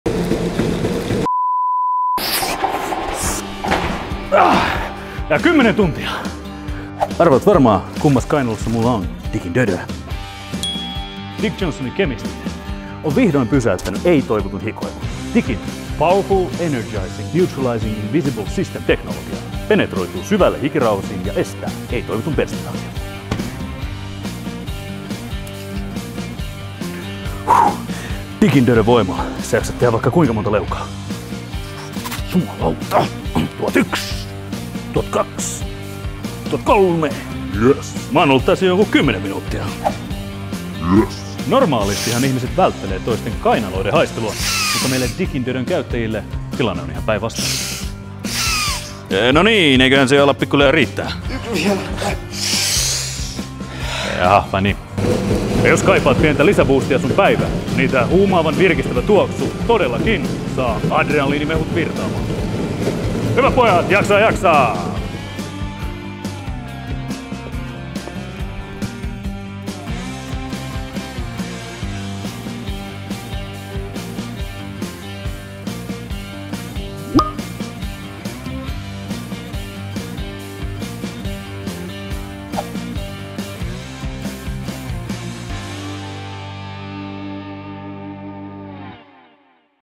Dick Johnson's Deodorant – huumaavan tuoksuinen dödö, joka ei petä!
Itsevarmuus multiploituu kun tietää, että tuoksuu ja näyttää hyvältä. Enivrant on päihdyttävä dödö, joka tuoksuu juuri sopivan miehekkäästi viskille ja vaniljalle. Hien- ja p*skanhajulle tulee välitön stoppi, kun deodoranttia levitetään puhtaaseen kainaloon. Erittäin riittoisa ja pitkäkestoinen dödö sopii jokaiseen päivään, erityisesti treffeille!